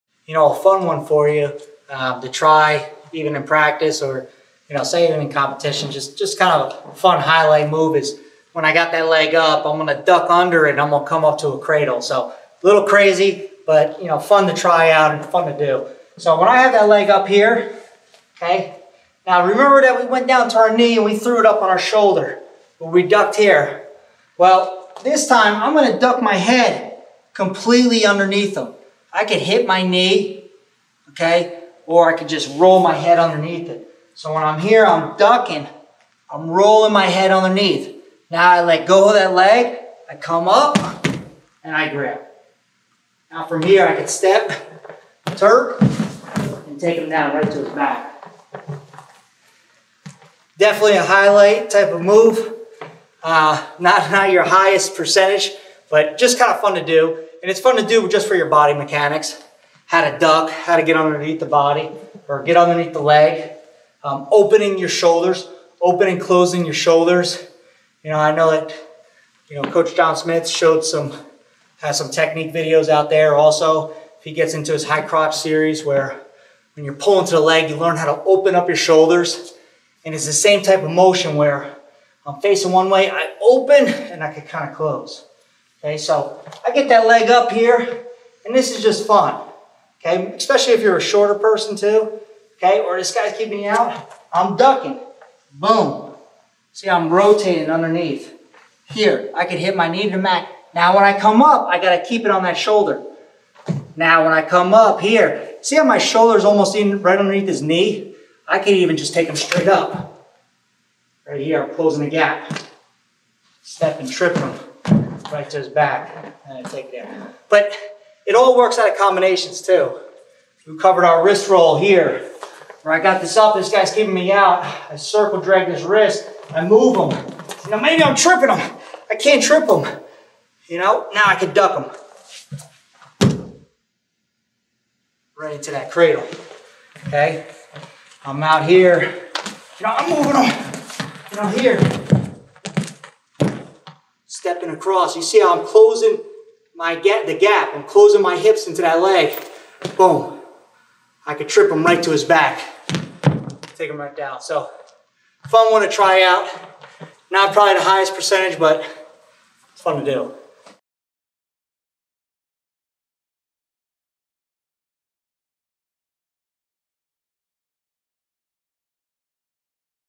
Single Leg Duck Finish to Standing Cradle by Zack Esposito
Oklahoma State Associate Head Coach Zack Esposito teaches a dynamic single leg finish with a standing front cradle